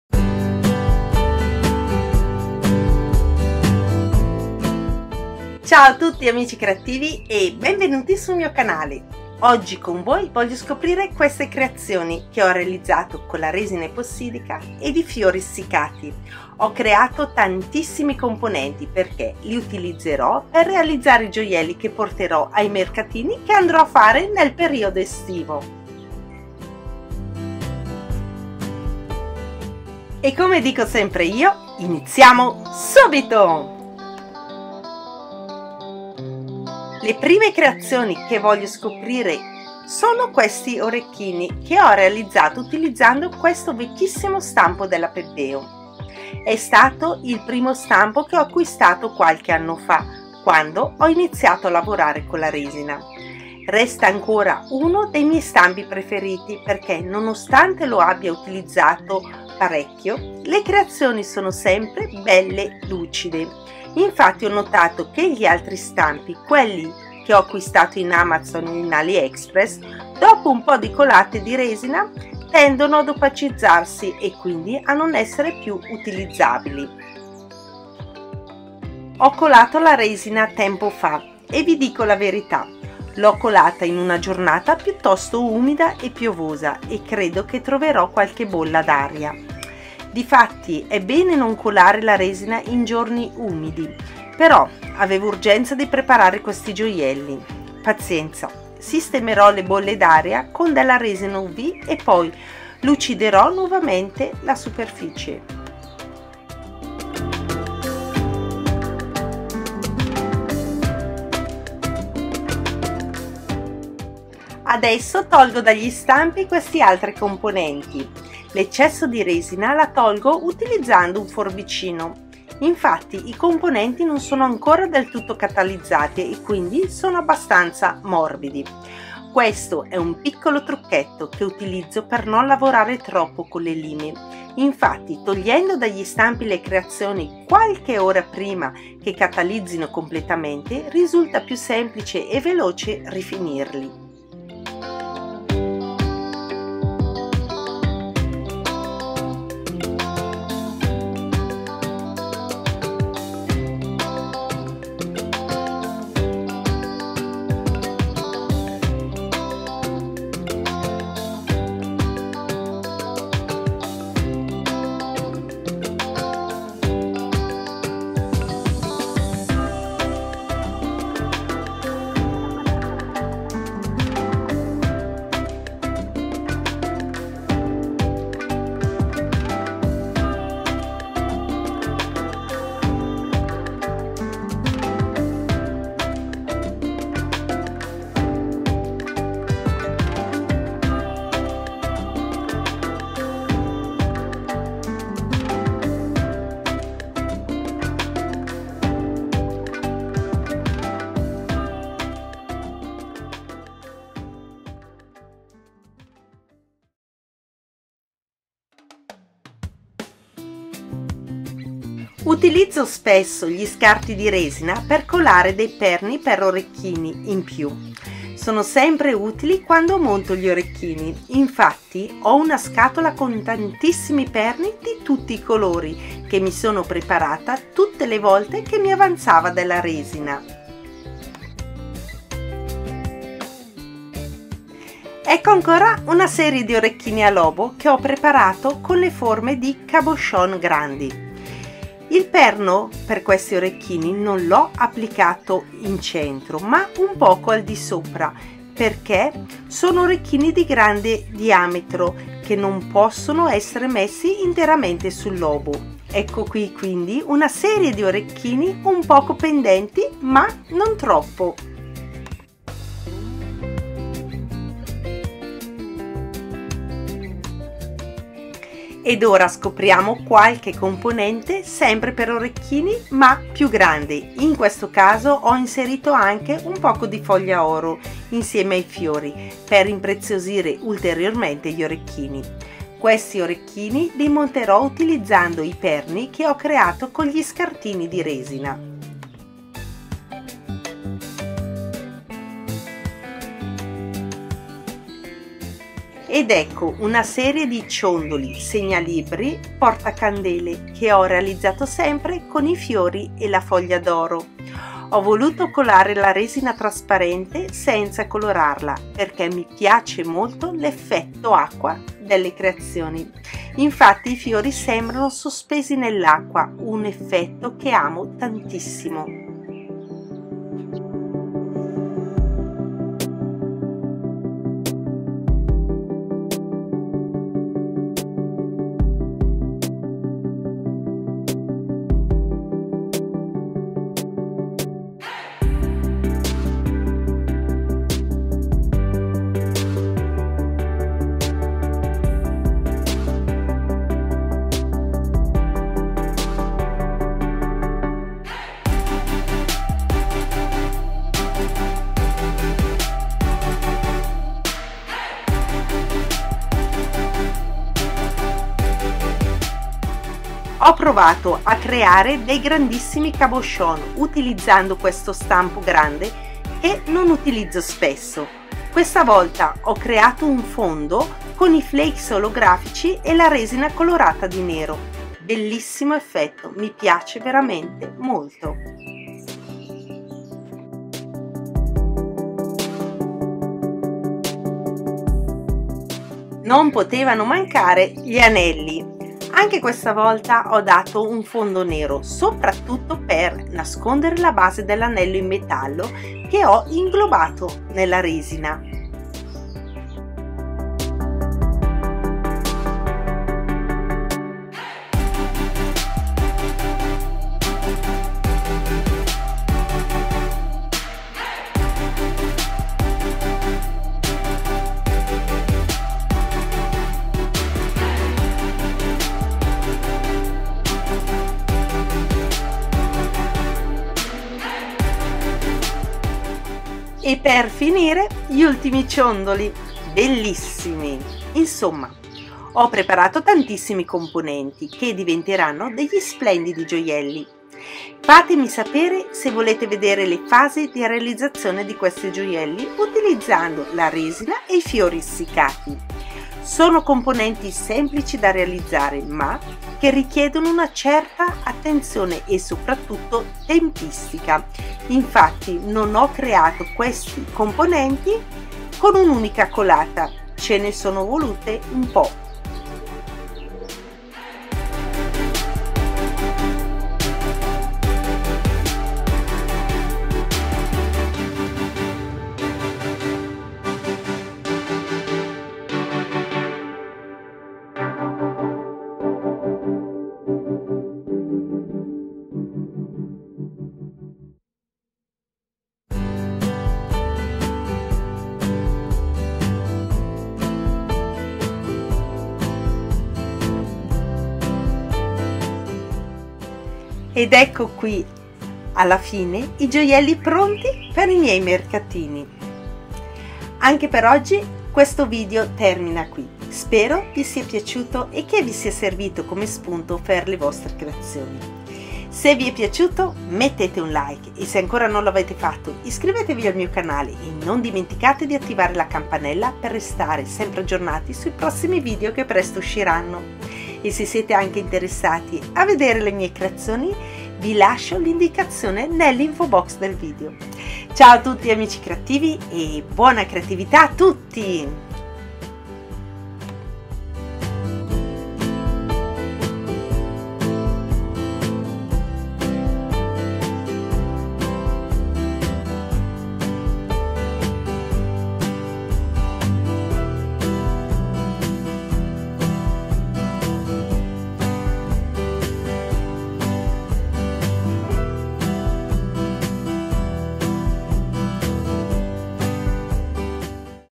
Scopriamo le creazioni in resina con i fiori
Ciao a tutti amici creativi in questo video scopriamo insieme le mie ultime creazioni che ho realizzato con la resina ed i fiori essiccati.

Per vedere le mie creazioni  visita il mio gruppo Facebook

seguimi anche sui miei social